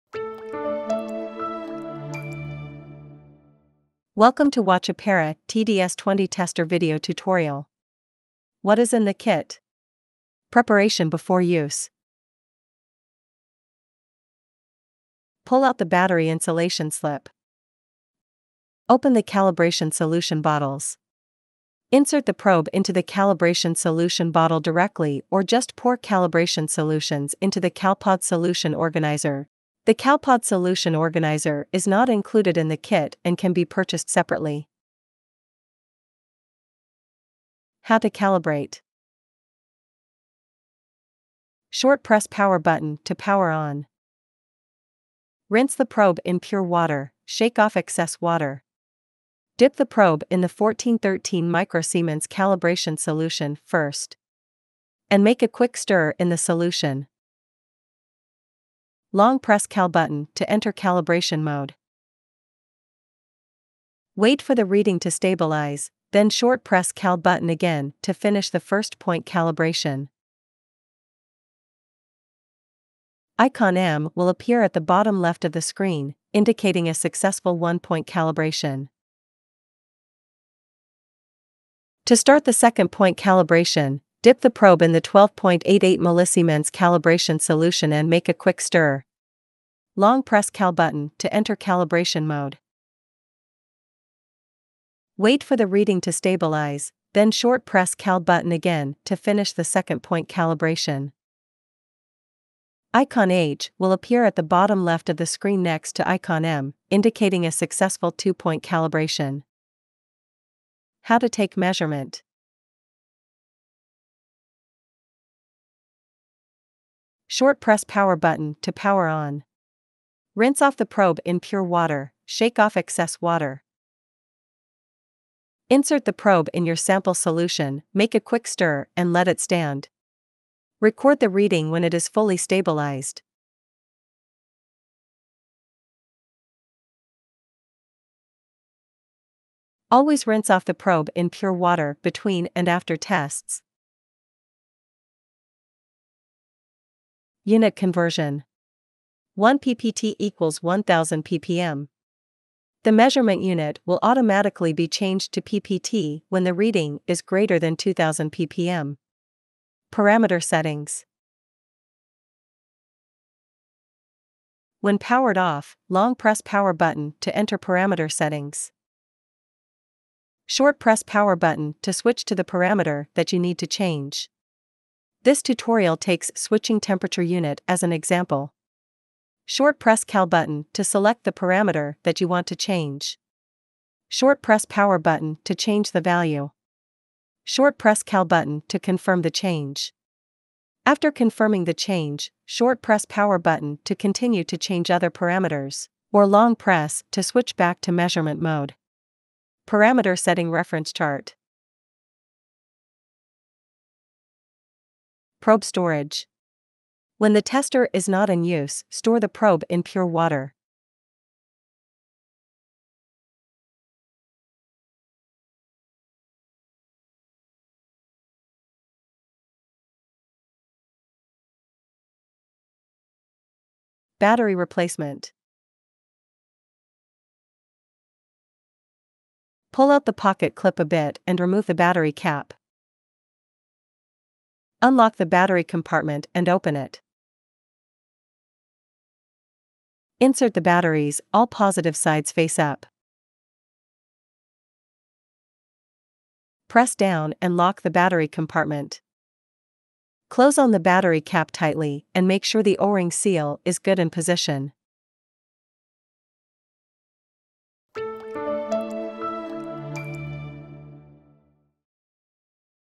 Apera Instruments TDS20 TDS Tester Tutorial
Apera Instruments TDS20 Pocket TDS Tester Tutorial
00:05 What's in the kit
00:11 Preparation before use
00:44 How to calibrate
01:52 How to take measurement
02:33 Unit Conversion
02:41 Parameter Settings
03:24 Probe Storage
03:45 Battery Replacement